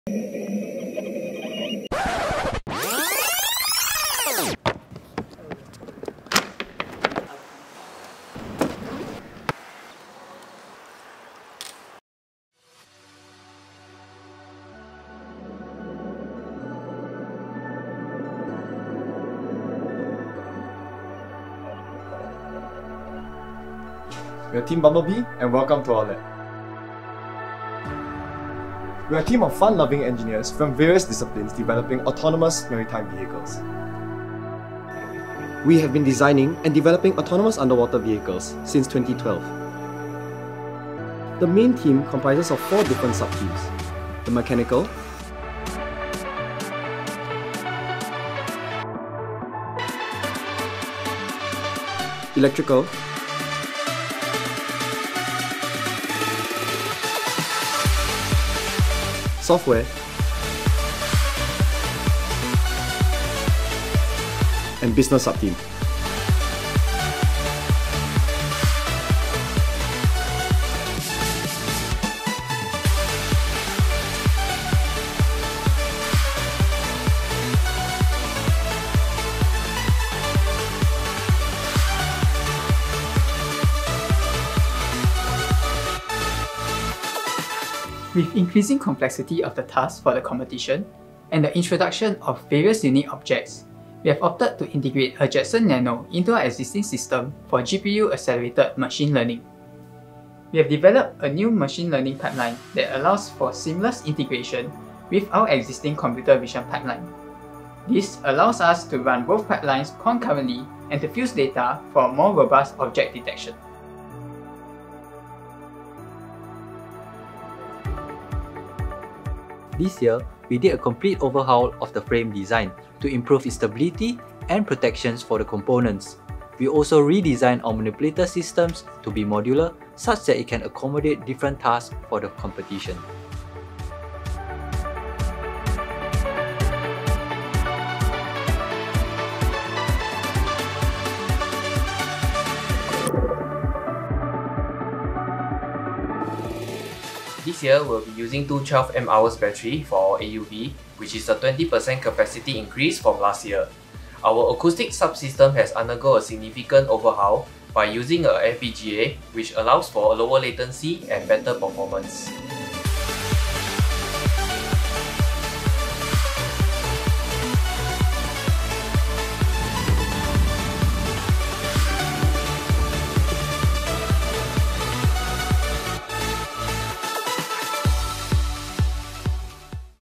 Bumblebee - RoboSub Team Video 2019
Bumblebee is a team of undergraduates from the National University of Singapore. This video was made for RoboSub 2019, introducing our members, along with some of the updates on our AUV for the RoboSub 2019.